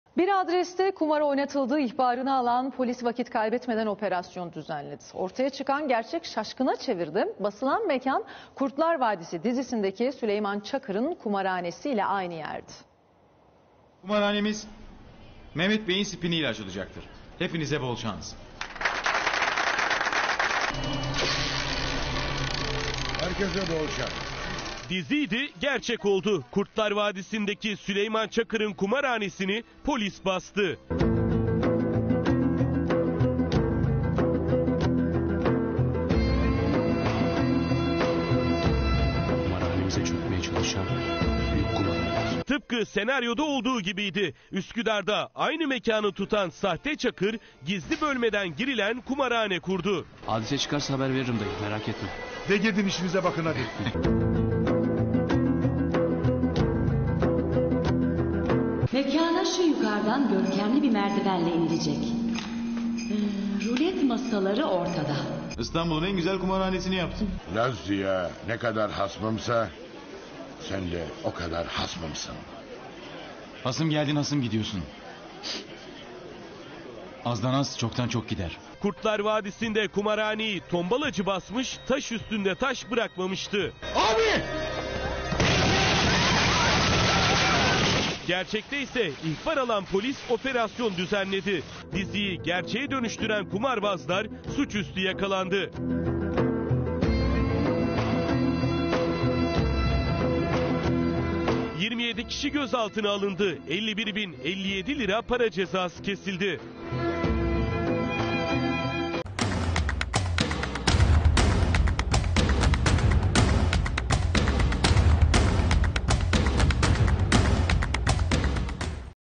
Çakır'ın kumarhanesi gerçek oldu
BİR ADRESTE KUMAR OYNATILDIĞI İHBARINI ALAN POLİS, VAKİT KAYBETMEDEN OPERASYON DÜZENLEDİ.. ORTAYA ÇIKAN GERÇEK ŞAŞKINA ÇEVİRDİ... BASILAN MEKAN, KURTLAR VADİSİ DİZİSİNDEKİ SÜLEYMAN ÇAKIR'IN KUMARHANESİYLE,  AYNI YERDİ..